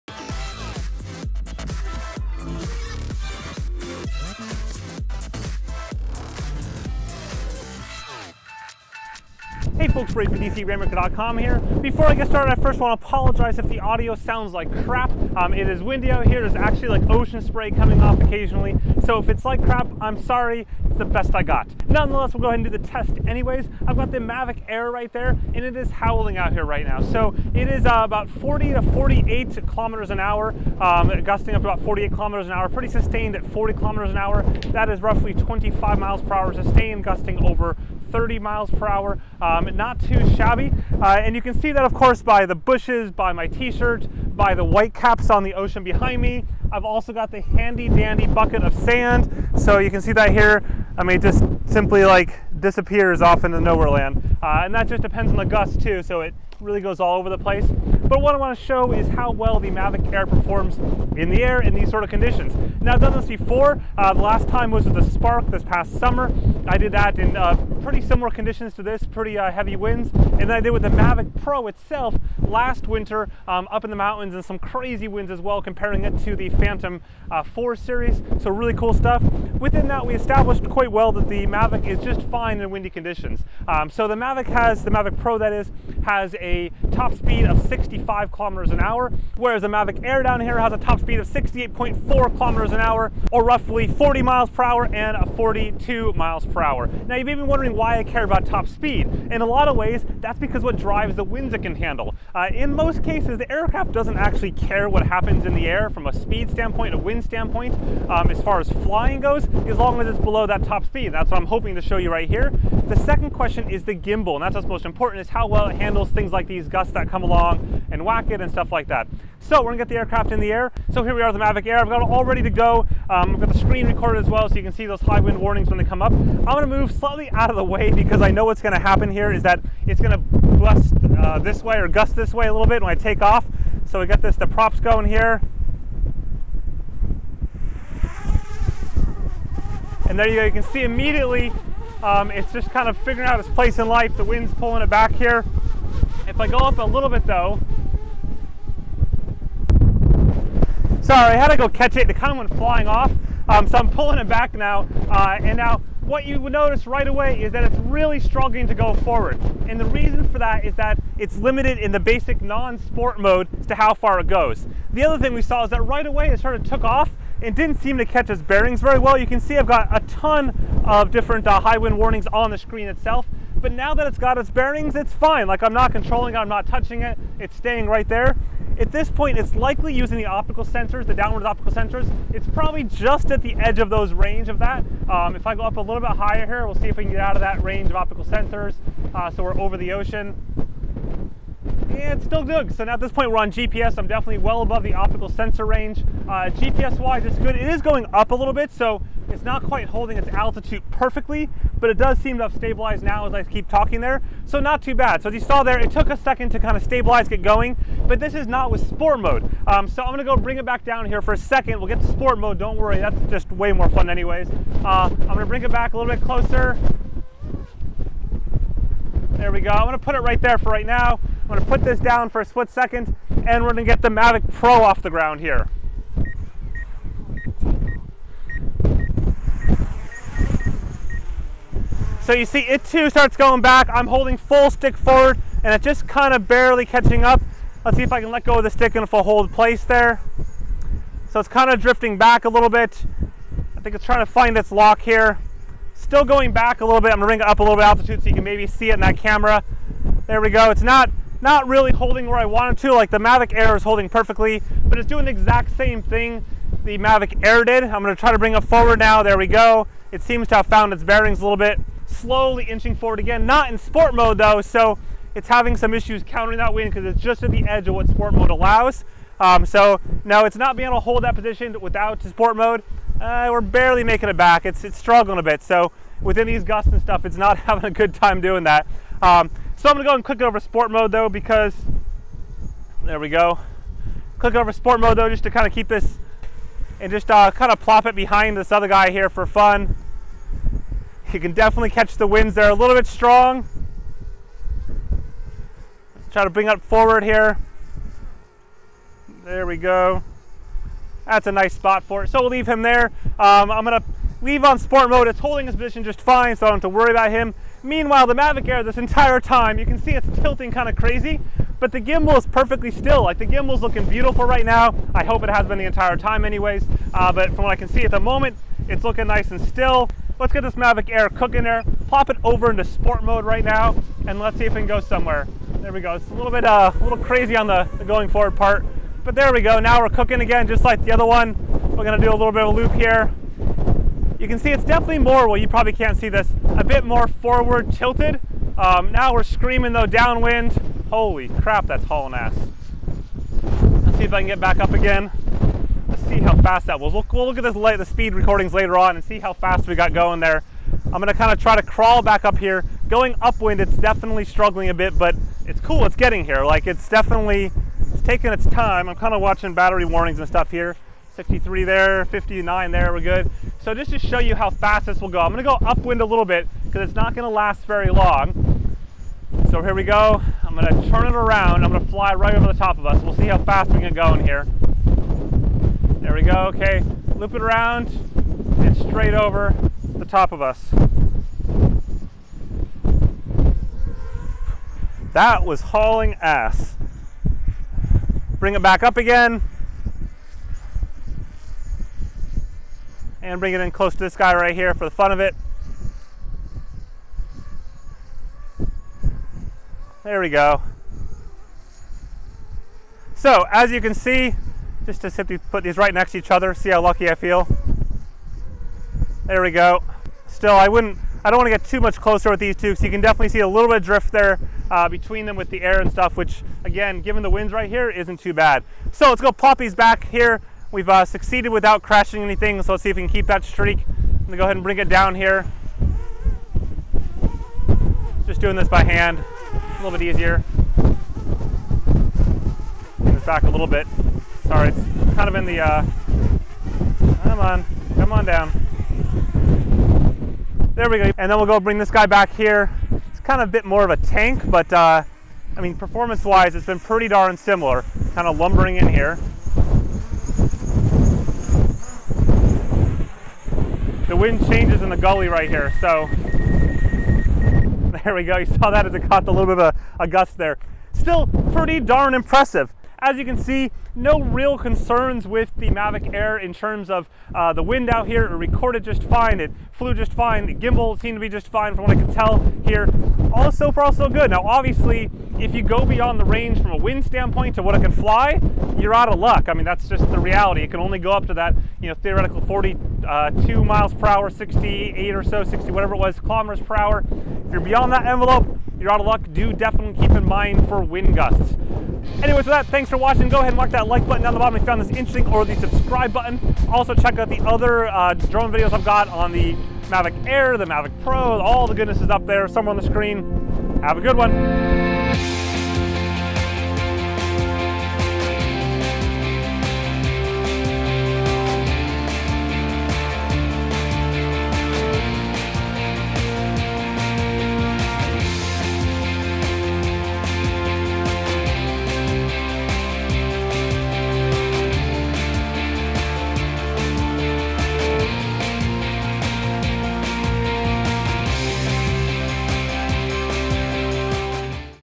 DJI MAVIC AIR: High Wind Tests!
DJI Mavic Air on Amazon

Just how much wind can the DJI Mavic Air sustain? I set out to find out.  On a day closing in on 50KPH (30MPH) winds - I push it to the limit.  Both in regular mode as well as sport mode, in 4K/30FPS as well as 1080P/120FPS.  Plus testing of how well it responds, takes off, lands, and more!  I also compare it to the DJI Mavic Pro too!

Mavic Air

Main Camera
The Drone
The Gimbal
Action Cam